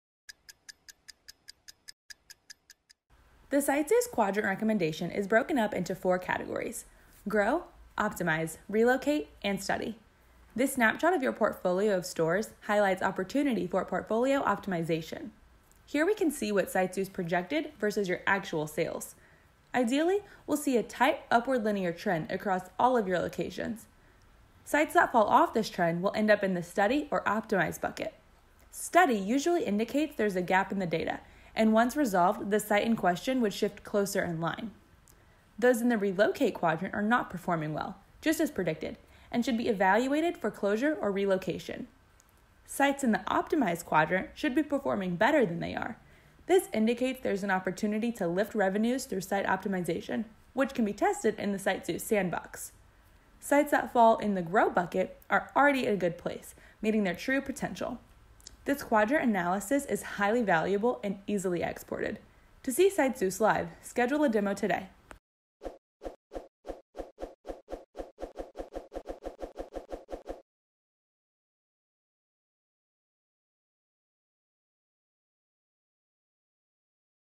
Under 60 Seconds: Evaluate your Portfolio for Optimization
Quickly analyze your entire portfolio's performance for fast and accurate location-based decisions.

The quadrant recommendation box in SiteZeus plots all of your sites into 4 categories based on the relationship between their actual and projected sales revenue. In seconds you can gain insights on which sites should be studied, relocated, grown, or optimized.